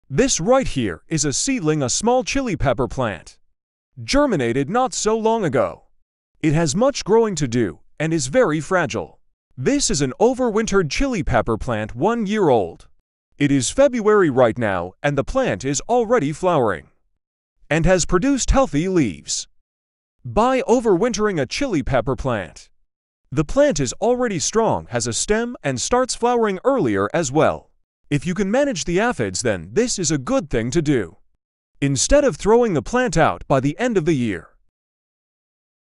Why you should OVERWINTER chili pepper plants
Each year many gardeners choose to throw out their chili pepper plants, and restart again next year. This is a short visual video to show you why you should reconsider throwing out you perfectly healthy chili pepper plant and instead overwinter it to next year

Aphids are a very common gardening problem especially when growing with chili peppers. There are methods to get rid of them, but sometimes people just get rid of the whole plant due to frustrations (which I totally get!)

When you overwinter a chili pepper plant, you don’t have to start all over again and care for a new plant from the beginning, but you already have a strong plant which you now produce a chili pepper variety that you like and want more of.

A thing to consider is overwinter the chili pepper plants that you like and start sowing new varieties on the side, instead of just sowing them all from the beginning.

Hope you enjoyed the short video, I’m terribly busy at the moment, but will try to make more chili and gardening content in the future